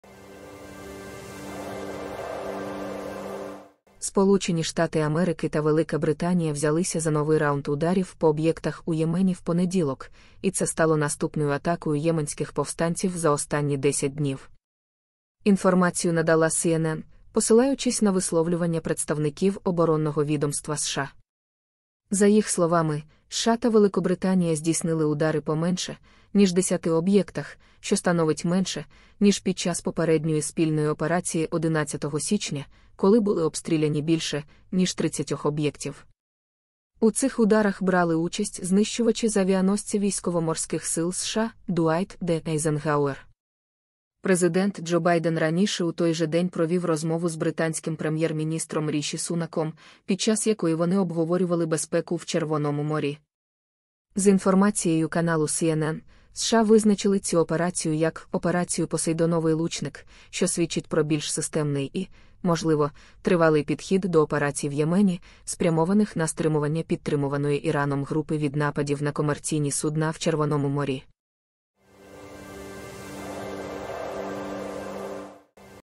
США і Британія завдали нових ударів по об'єктах у Ємені – CNN
USDT: TSJfcHW5UBth2qsR6jYUeGsxJDYYL16YCL

Bitcoin
BTC: 1PYFjXGCBvzyjpbuPRiy1P7DwndWNcTKfG